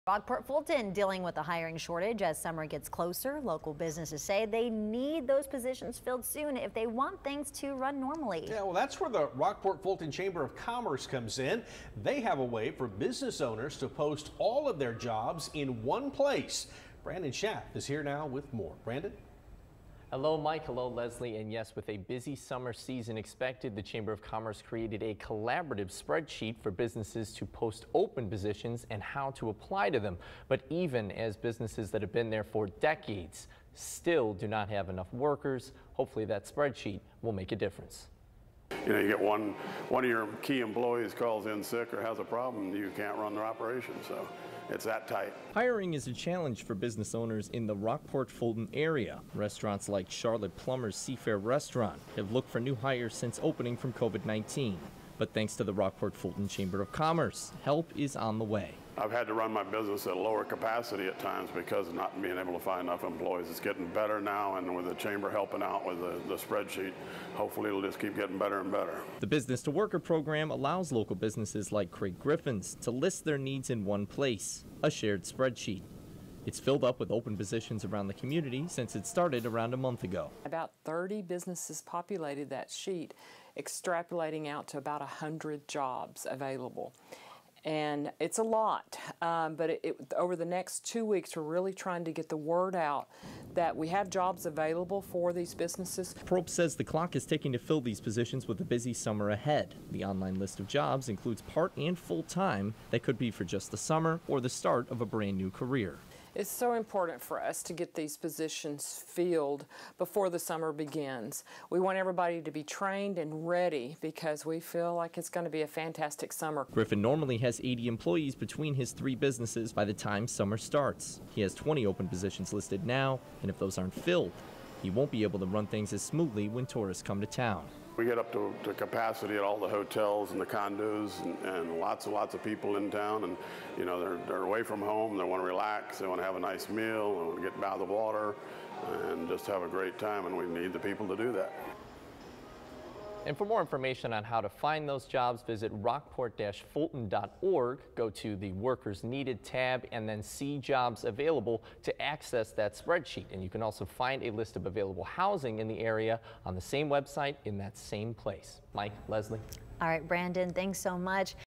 Rockport-Fulton looking for new hires ahead of summer tourism
The Business to Worker Program allows local businesses like Craig Griffin's to list their needs in one place